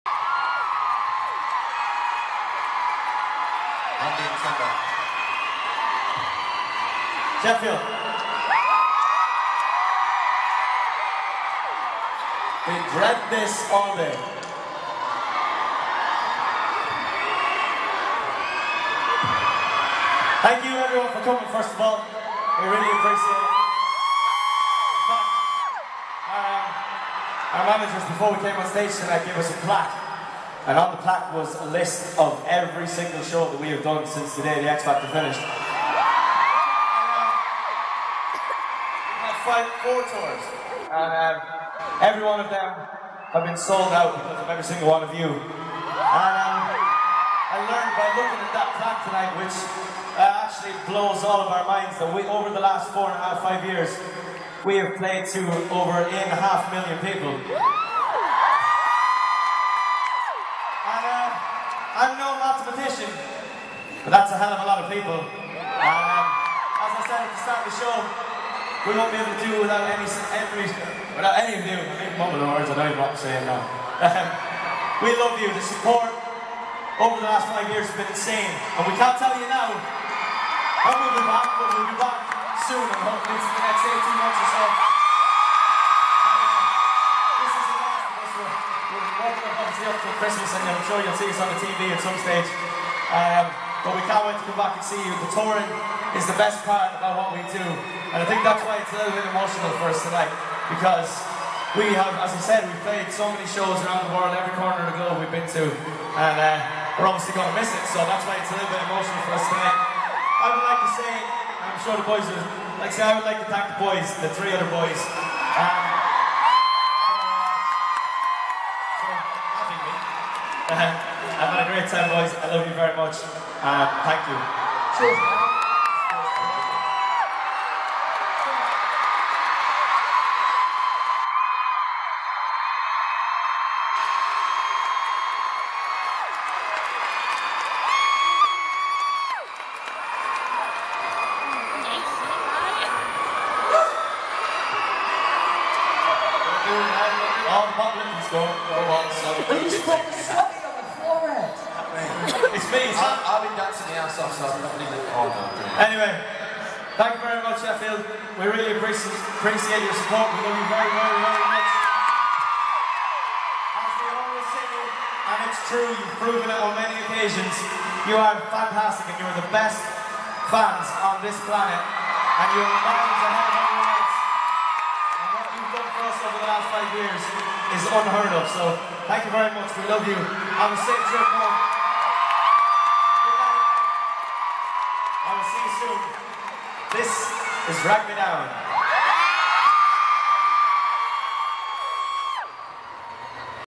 Niall's Speech & Group Hug Sheffield 31.10.15
One Direction's last night of the OTRA Tour.